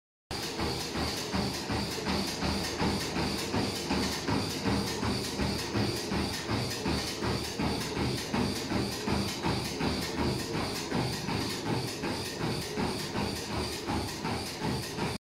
CardioStrong TX30 von Sport-Tiedje nach "Reperatur" eines Technikers
Übrigens.. das Laufband in dem Video ist bereits ein Austauschgerät, weil das erste Laufband ebenfalls diese Geräusche gemacht hat.